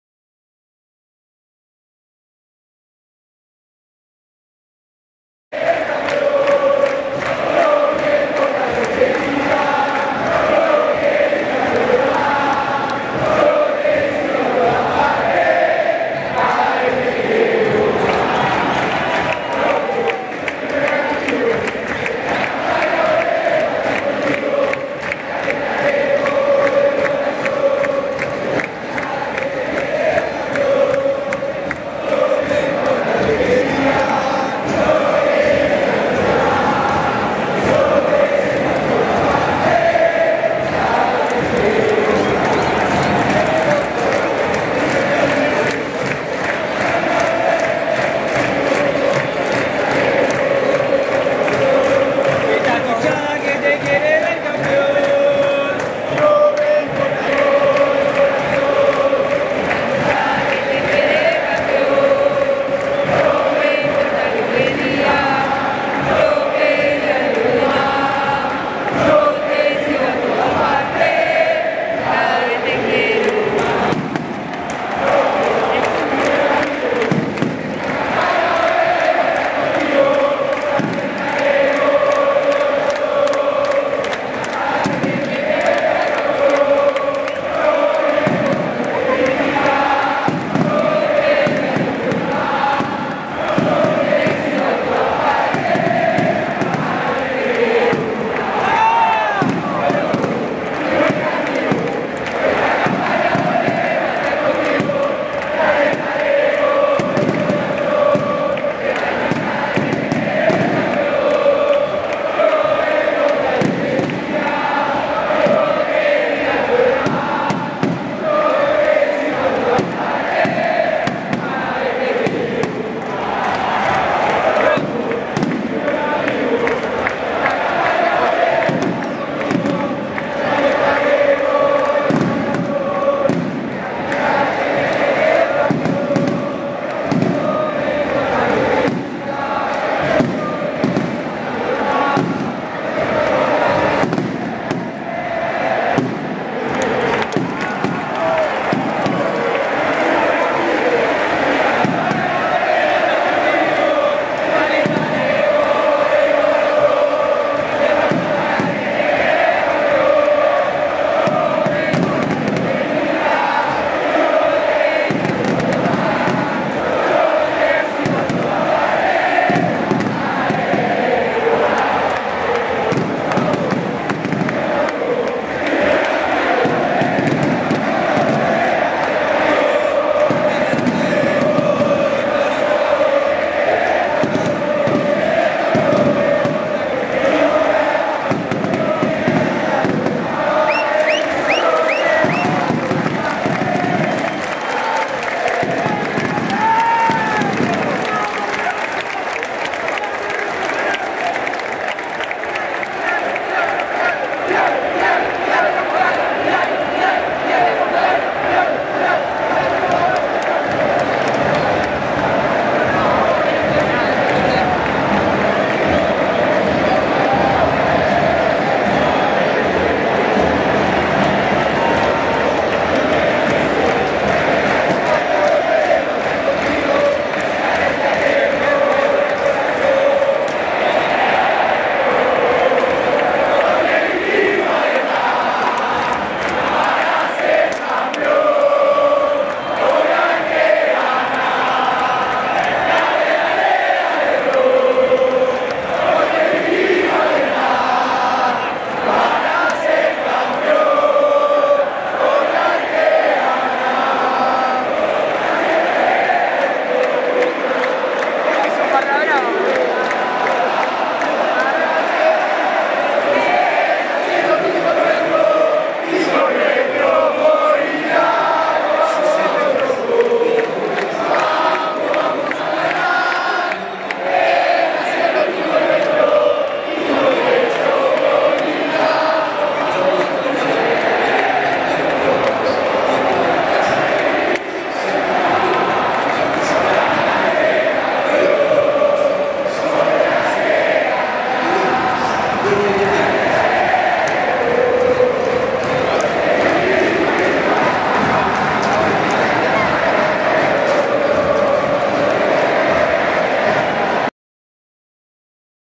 Salida de Independiente en el ClásicoDeAvellaneda
Salida de Independiente en el Clásico de Avellaneda por la séptima fecha de la Copa de la Liga 2024